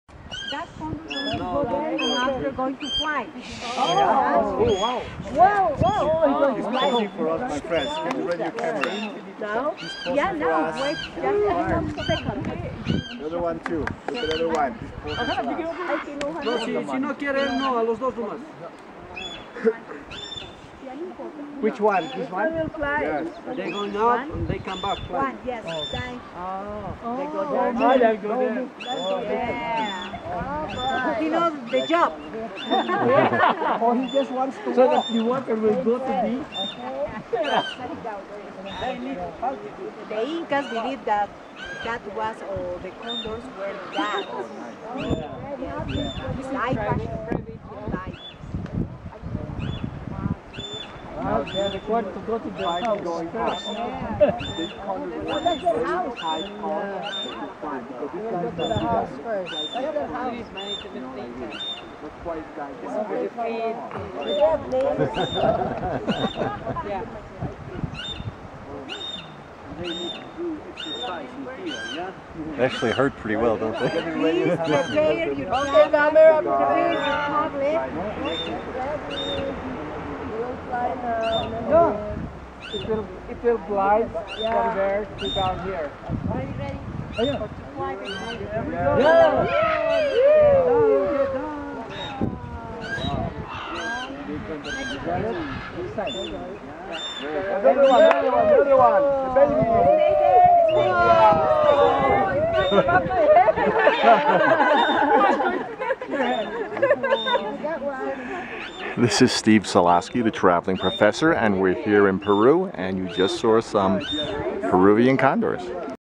Peru Condor Preserve
Cochahuasi Animal Preserve, near Cusco Peru has a wide variety of endangered species.  for more travel videos